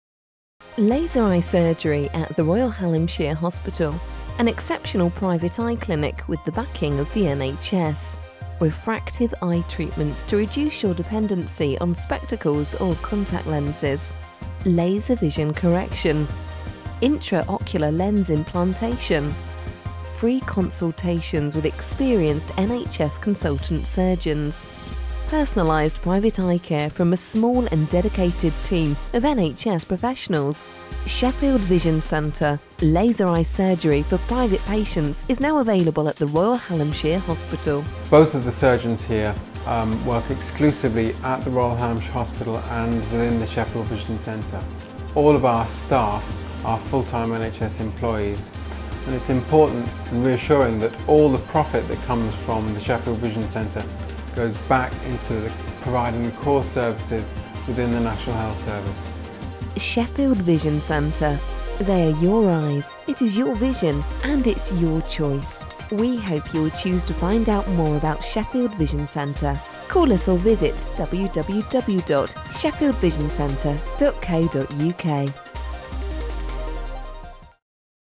Welcome to Sheffield Vision Centre
Refractive Surgery (Laser Eye Surgery or Intra Ocular Lens Surgery) can be a tremendously positive and life-changing experience.  At Sheffield Vision Centre we offer you a free assessment with a consultant Ophthalmologist who will examine your eyes and discuss in detail, your needs and your visual expectations.